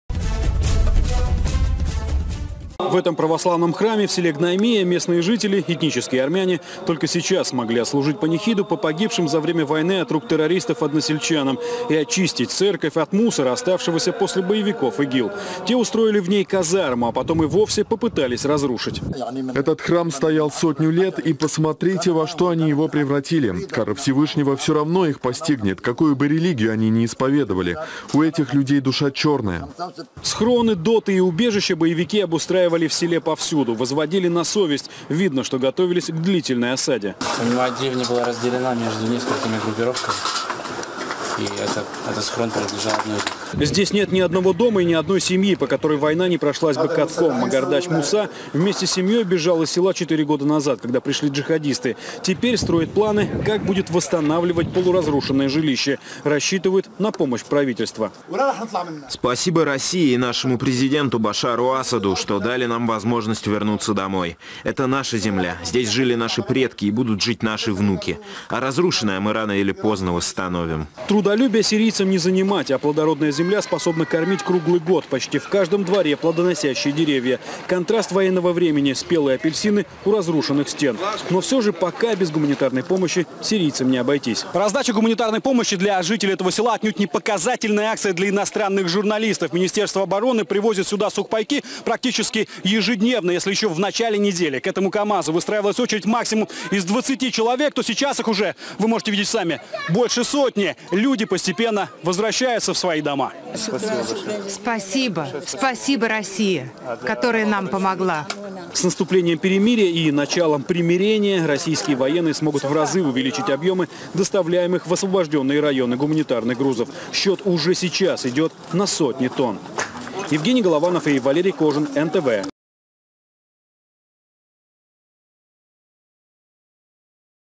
В освобожденные от террористов ИГИЛ регионы Сирии возвращается мирная жизнь Новости Сирии Сегодня
Самые новые,свежие новости!